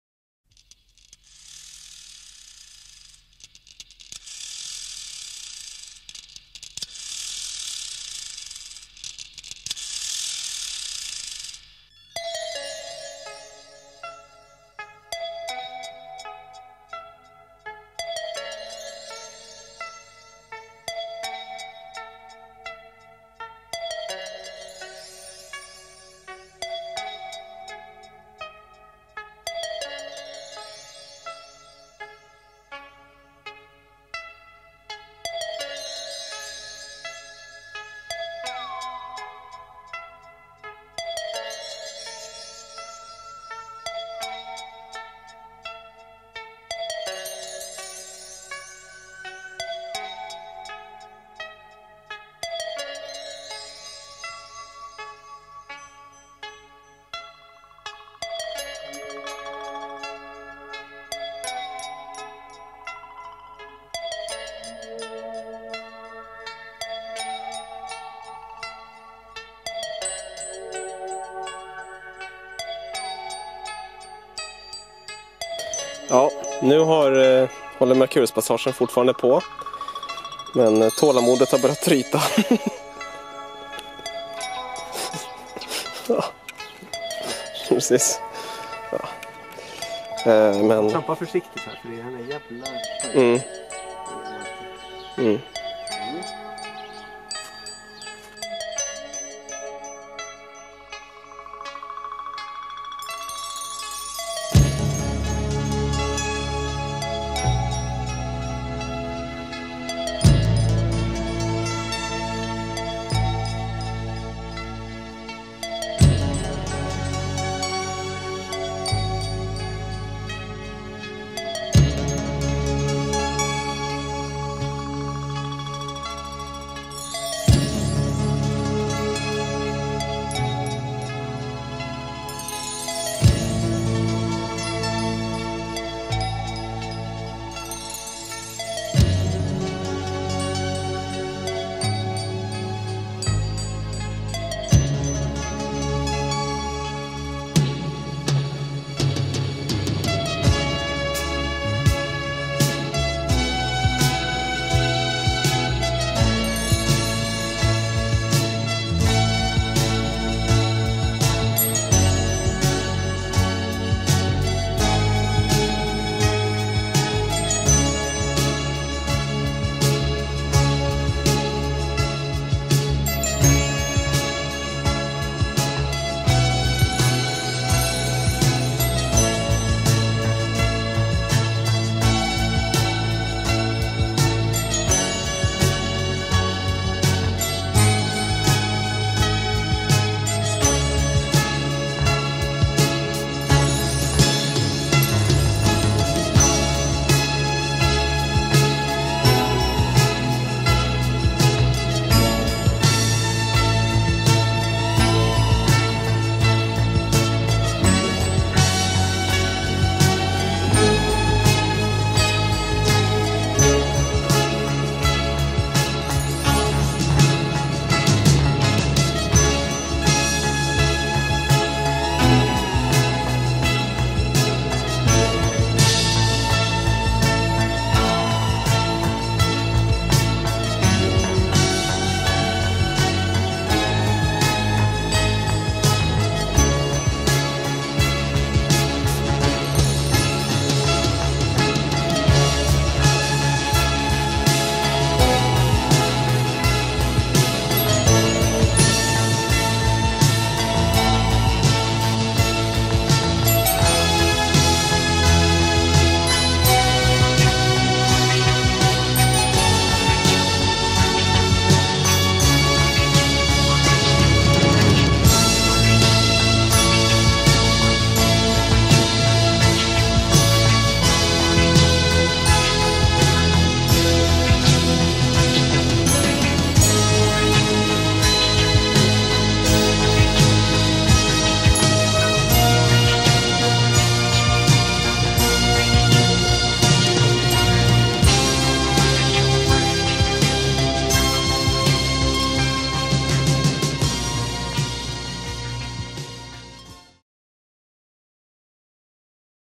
Transit of Mercury
This shows the transit of Mercury on the 9'th of May 2016.
For this a h-alpha telescope was used, which shows light in the wavelength of around 656nm.
A frame was captured every five minutes.